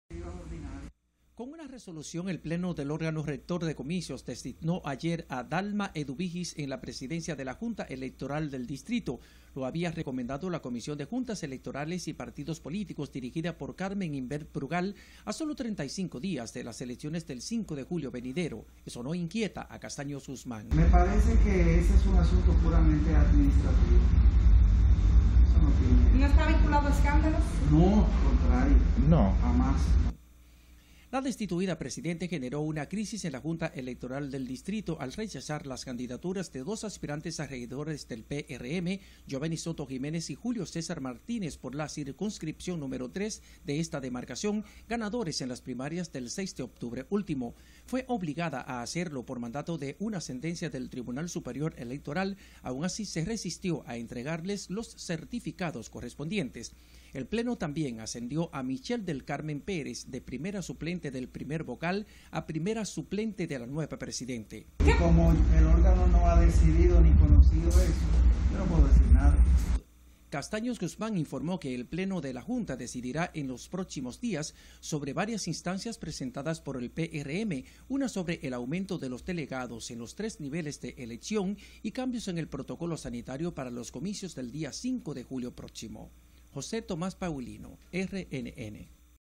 Castaños considera “rutinarios” cambios en JCE del DN
El presidente de la Junta Central Electoral (JCE), Julio Cesar Castaños Guzmán, consideró como un proceso rutinario la destitución de la titular del organismo en el Distrito Nacional, Marie Linette García Campos.